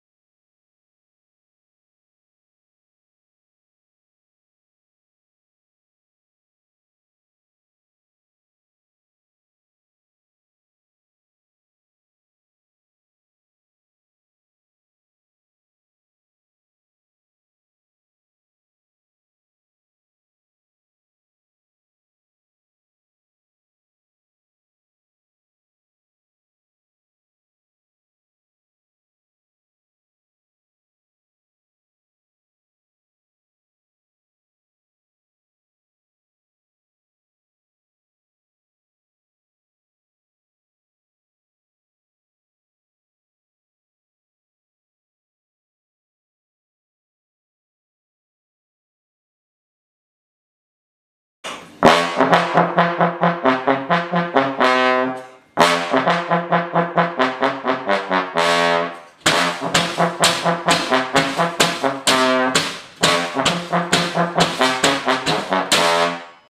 When Jack Isn't Home
This what Timothy and Toby do when Jack is not home (XD)